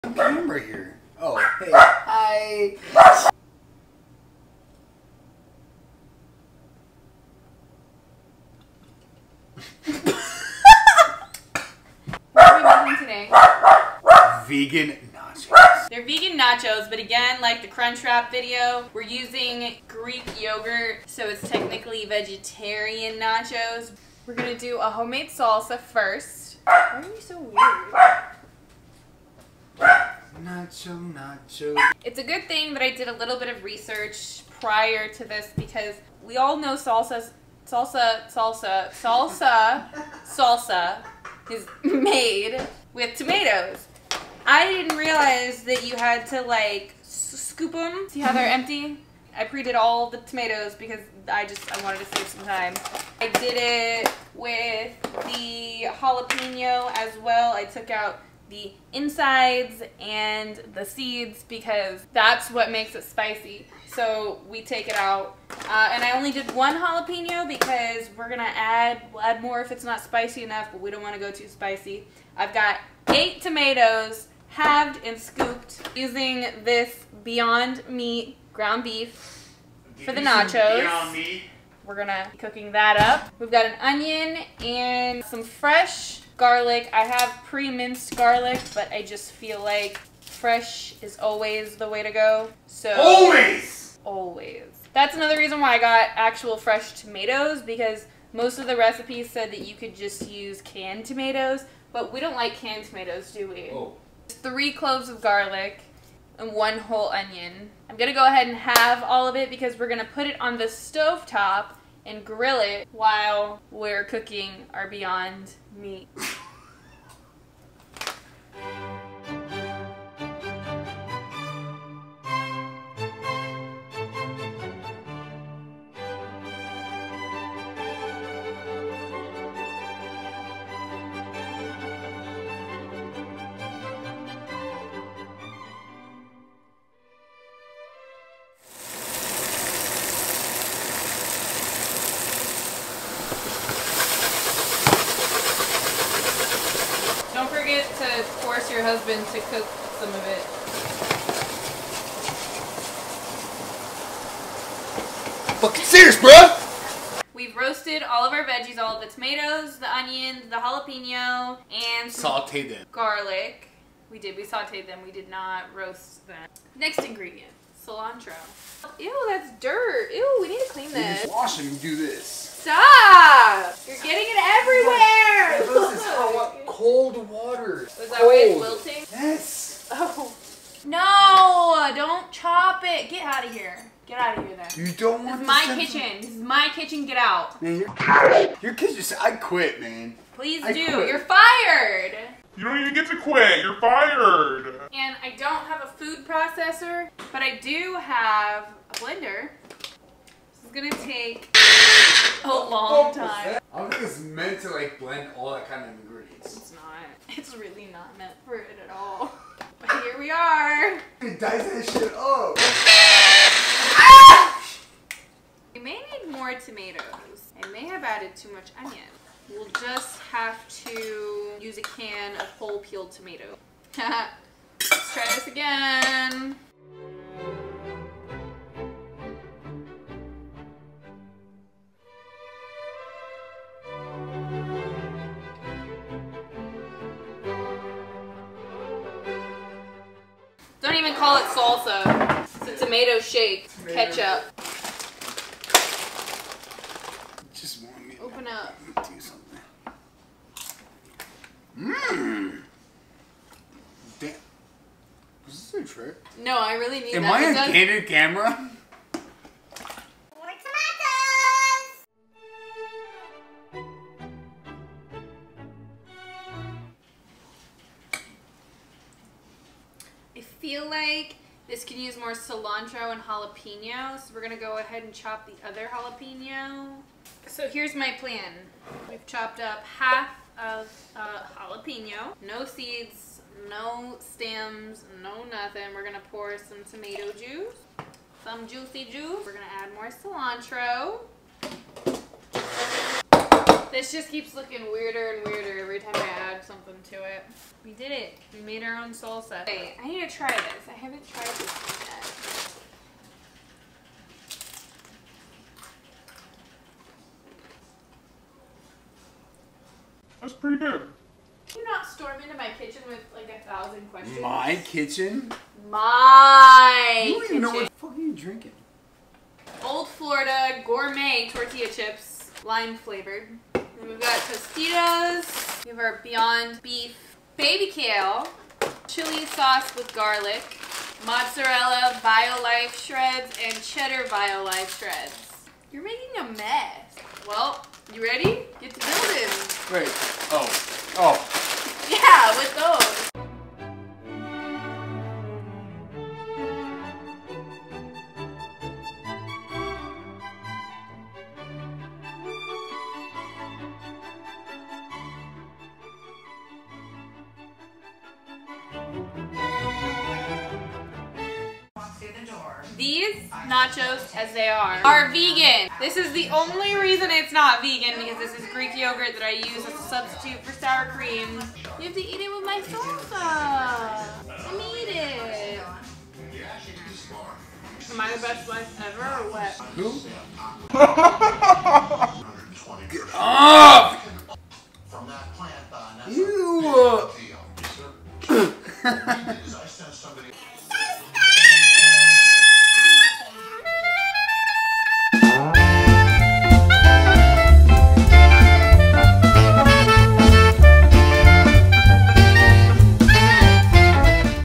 Tipsy Kitchen | Vegan Nachos with Homemade Salsa
I think my co-host was tipsier than me 😆
My first attempt at homemade salsa, and I didn't have the proper equipment 🙃